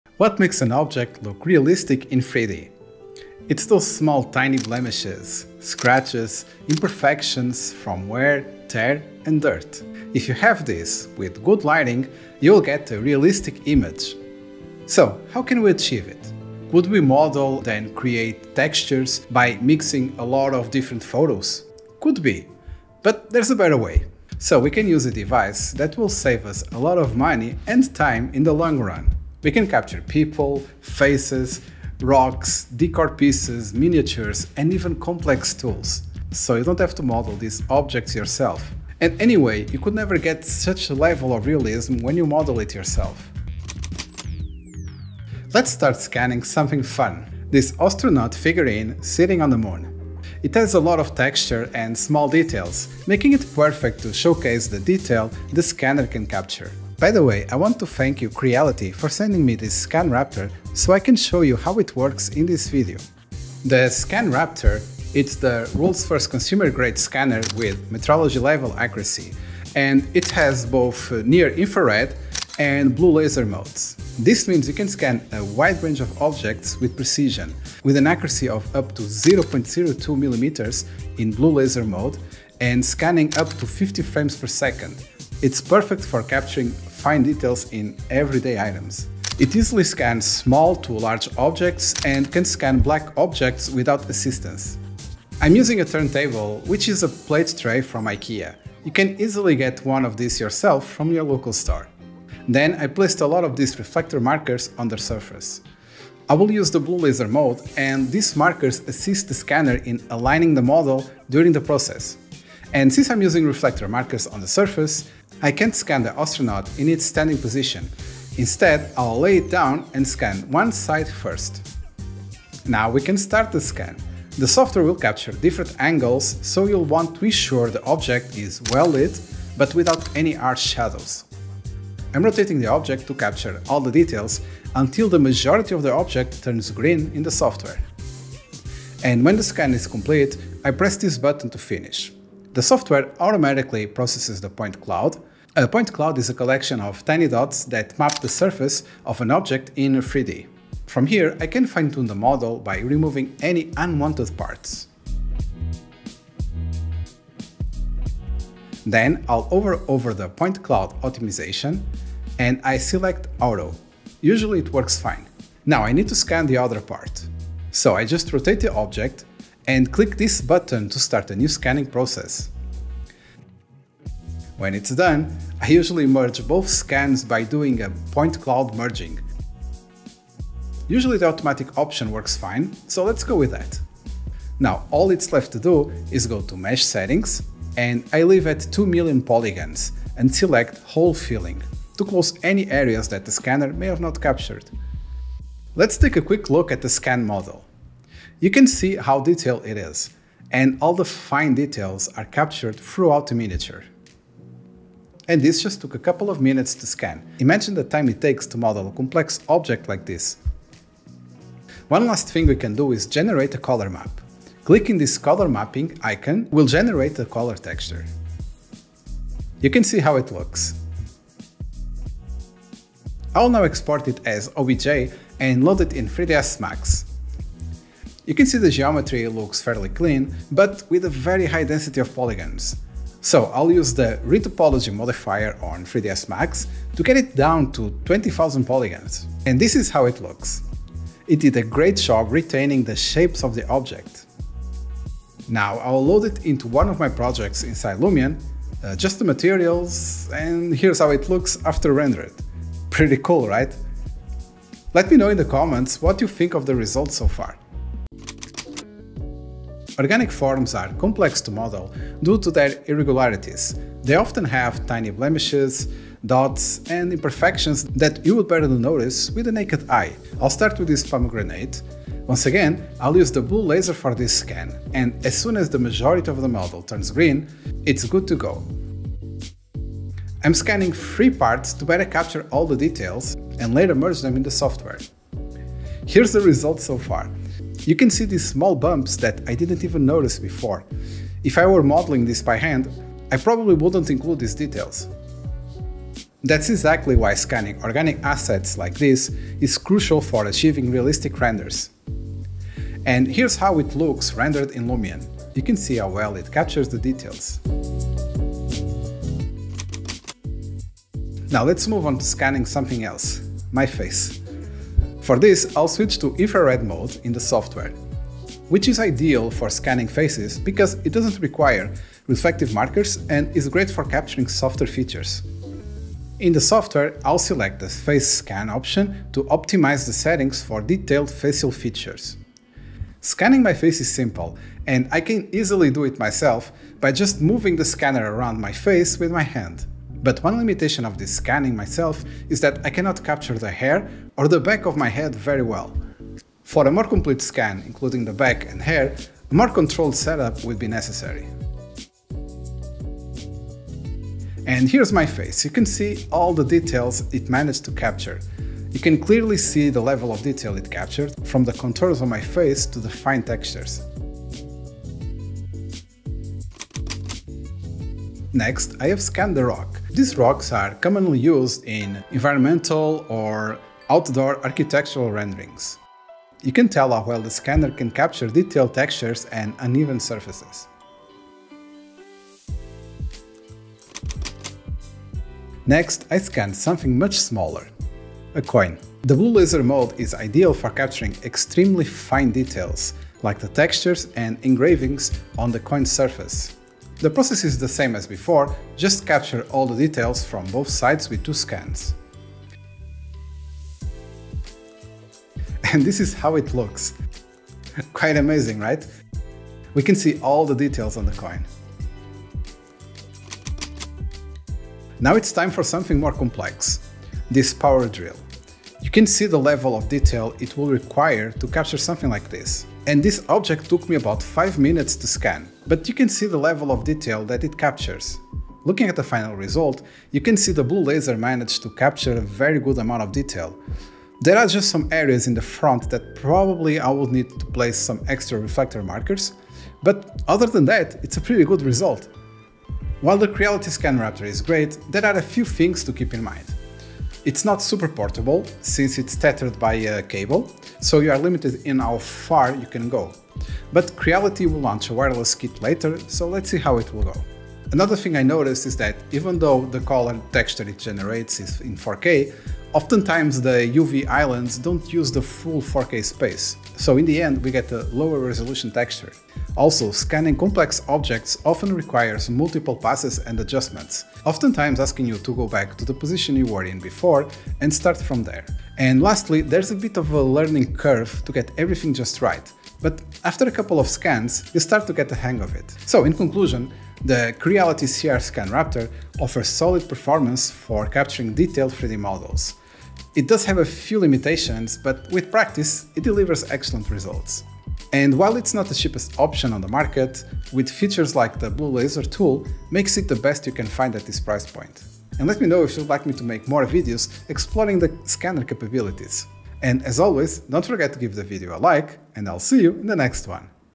The Trick Behind Hyper-Real 3D Models – No Modeling Needed
Unlock the secret to achieving stunning realism in 3D modeling and rendering!

In this video, I demonstrate how to use the Creality CR-Scan Raptor to bring everyday objects into the digital world with unparalleled detail. Whether you’re scanning complex tools, organic items, or even your own face, this scanner makes it faster and easier to create ultra-realistic 3D models.

*What You’ll Learn*:
How to capture intricate details using blue laser and infrared modes.
Scanning various objects, including power drills, coins, and plants.
Post-scan optimization tips, like retopology and material adjustments in 3ds Max and Lumion.
The pros and cons of using the Creality CR-Scan Raptor for 3D projects.

Objects Scanned:
▶️ A power drill to demonstrate texture and grip details.
▶️ An astronaut figurine, optimized for rendering in Lumion.
▶️ A coin for capturing ultra-fine engravings.
▶️ A pomegranate, showcasing realistic organic imperfections.
▶️ A face scan using infrared mode for capturing soft features.

VIDEO SUMMARY
In this video, I share how to make realistic images, by 3D scanning real objects.

*Why This Scanner Stands Out*:
With metrology-level accuracy and a resolution of up to 0.02mm, the Creality Raptor simplifies the process of creating high-quality 3D models. Plus, it’s a game-changer for ArchViz and creative 3D projects!